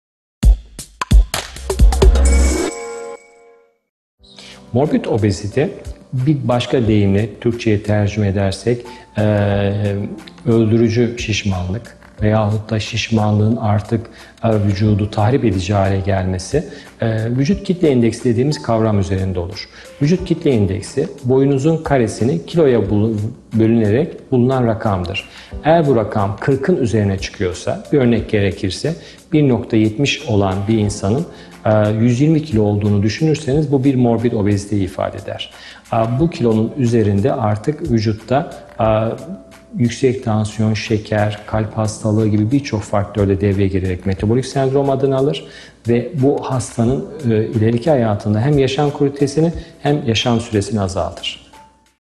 Morbid obezite nedir?
Morbid obezitede şişmanlık vücudu tahrip edici orandadır. Kişinin vücut/kitle endeksi 40'ın üzerindeyse morbid obeziteden bahsedilebilir. Morbid obezite hem yaşam kalitesini düşürür hem de  yaşam süresini kısaltır.